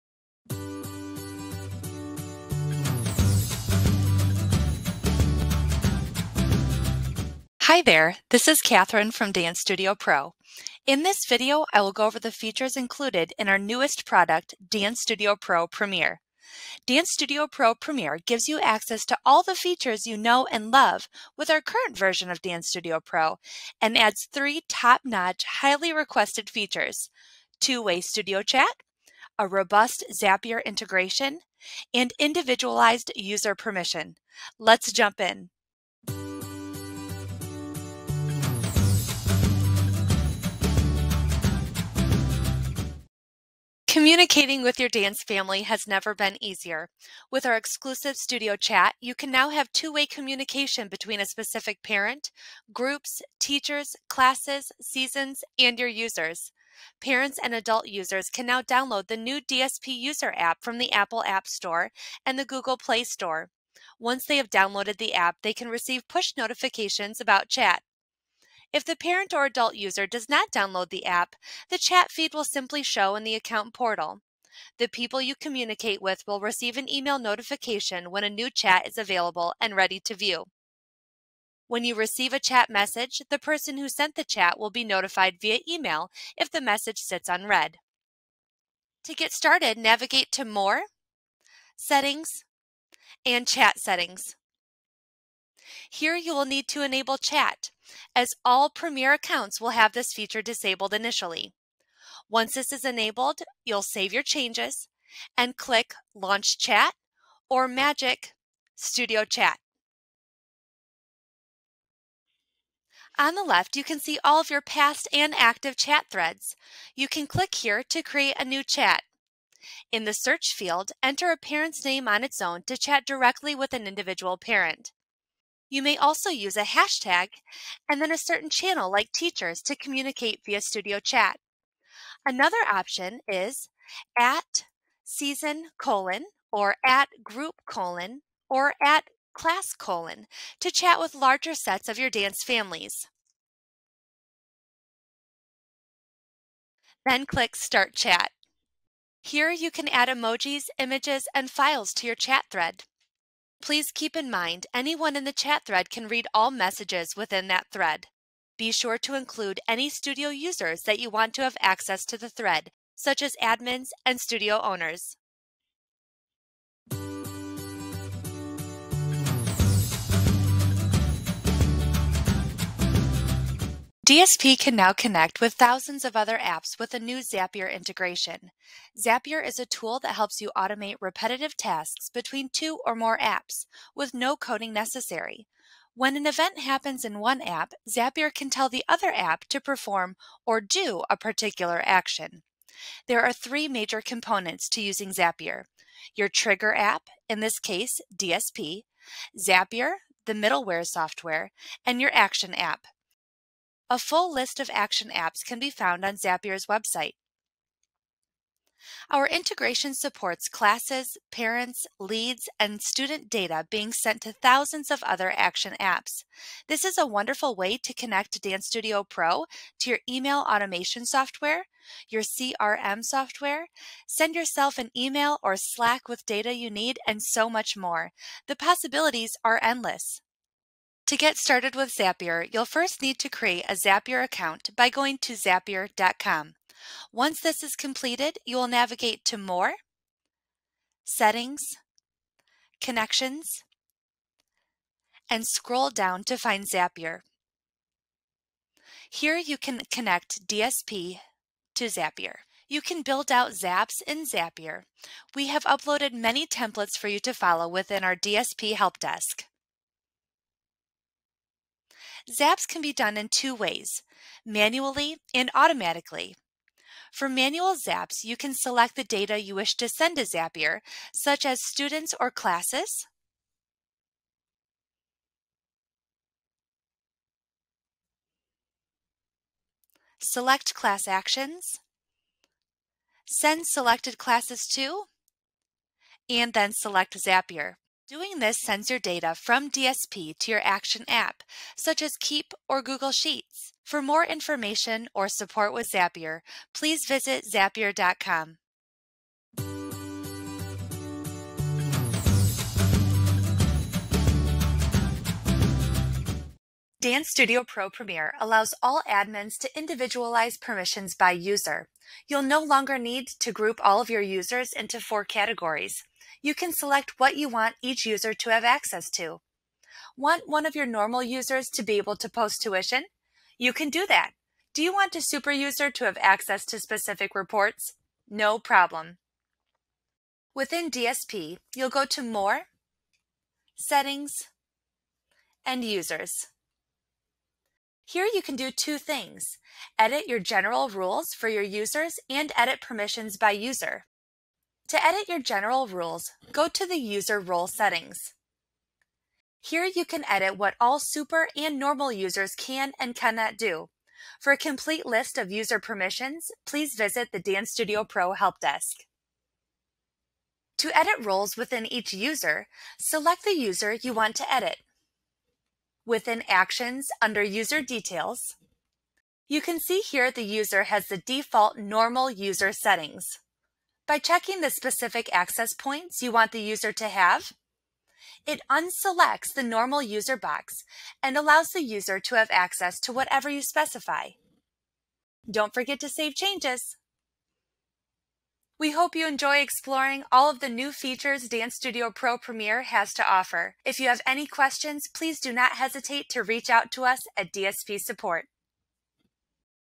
Introducing DanceStudio-Pro Premier
Introducing the latest upgraded version of our software, DanceStudio-Pro Premier!

This version includes all current DSP features along with:

- Studio Chat – Send and receive messages with parents using our brand new DanceStudio-Pro Portal App
- Individualized User Settings – Decide which features each staff member can access
- Zapier Integrations – Have DSP connect with all your favorite software